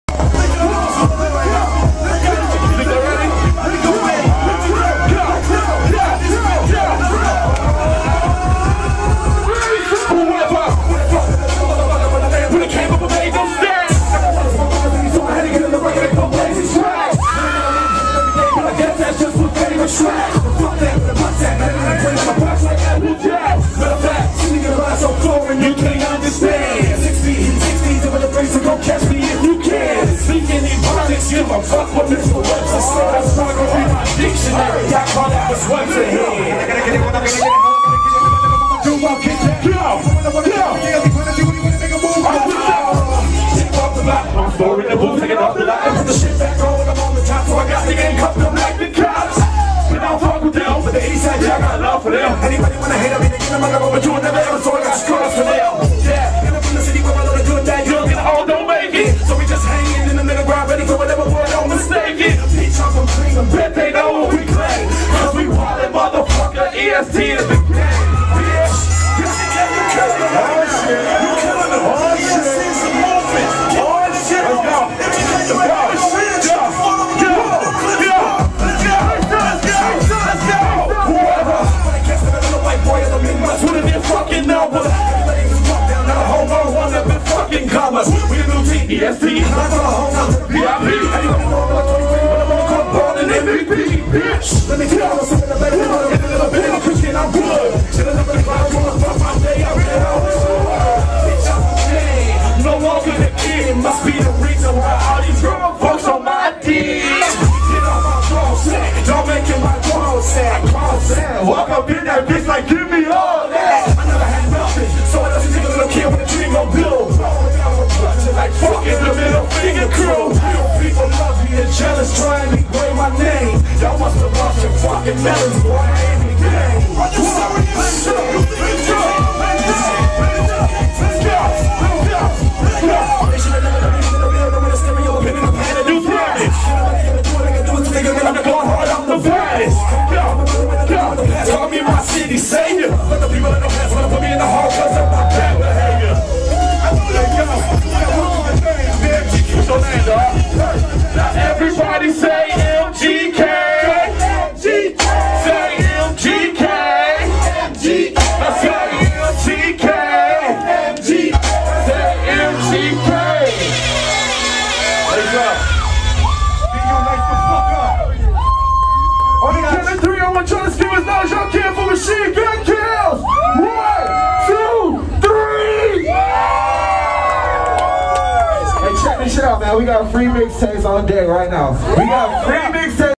Machine Gun Kelly at BU AnimalHouseTour 4-9-11
Machine Gun Kelly puts on a crazy hype show regardless of if there's 10 people in the crowd or 10,000.

4-9-11